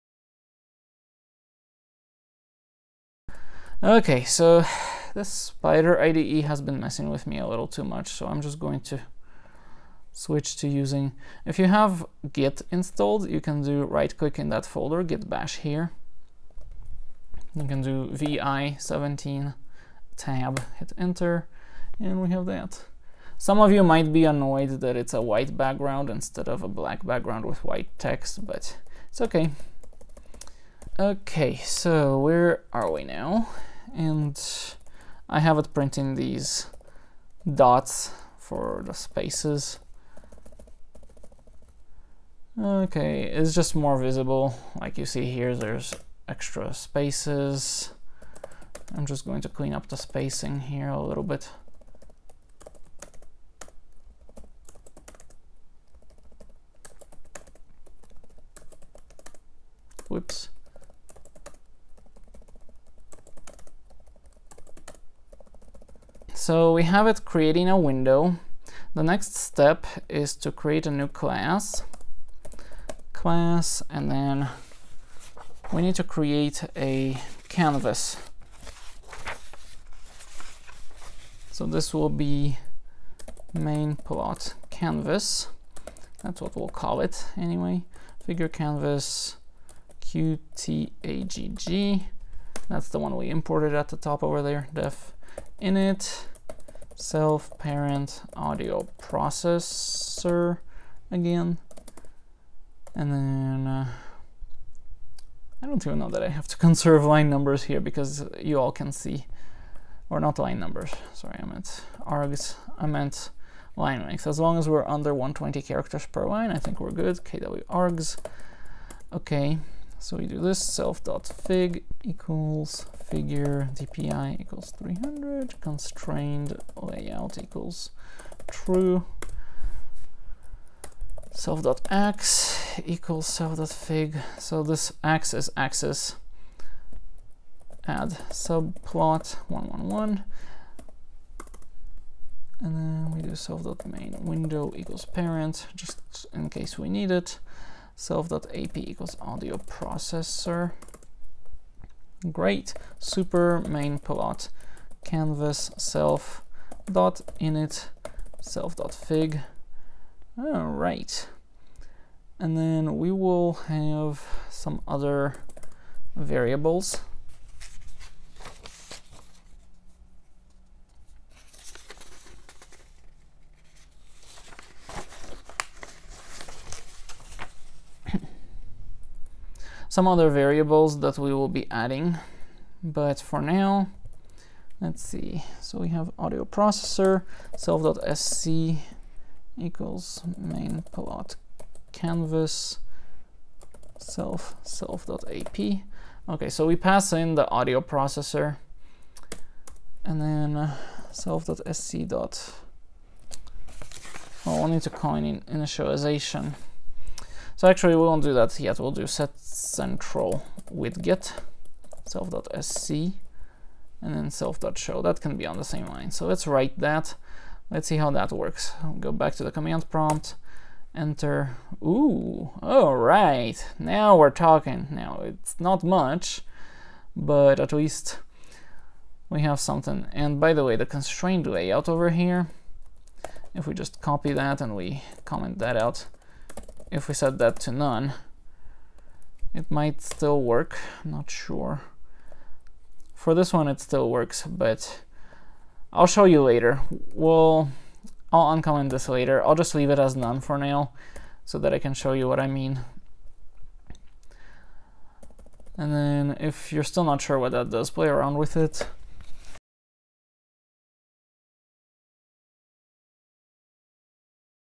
Part 5: Matplotlib canvas - Python PyQt5 GUI
This is a Python tutorial series for creating a GUI in PyQt5 and using matplotlib to add an interactive plot to it. The plot shows the sound spectrum from an MP3 audio file, and hovering the mouse over the plot makes a "harmonics ruler" show up, showing where harmonics might be located. Clicking the mouse plays the currently hovered-over pitch using the computer's speakers. The keyboard arrow keys allow for navigating through time in the MP3 file, and pressing the space bar plays the current audio segment, the spectrum for which is being shown in the plot.

As always, thank you for tuning in, and have a nice day! :)
- Mandy Software